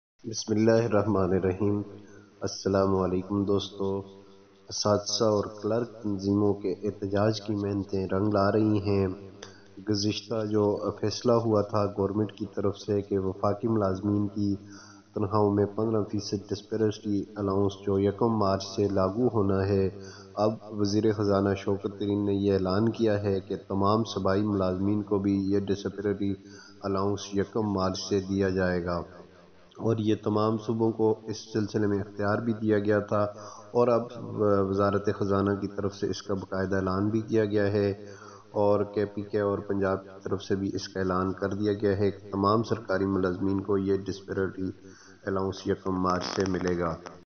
Latest Salary Increase News of Government Employees || Pay Increase Latest Update 2022 | Notice Issu
Latest Salary Increase News of Government Employees || Pay Increase Latest Update 2022
Latest Update about pay increase of government employees in 2022.
Finance Minister has announced to increase pay, salary of employees before 10th february 2022.
salary increase news 2022
pay scale revised
scale upgradation of government emploees.